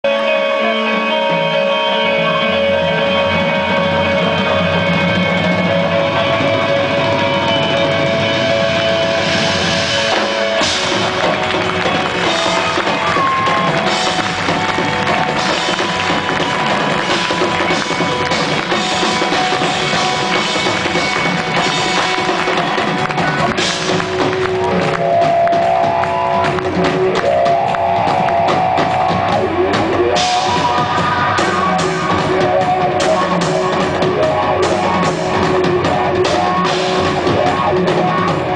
Blue Man Group
Blue Man Group performing in London, 2007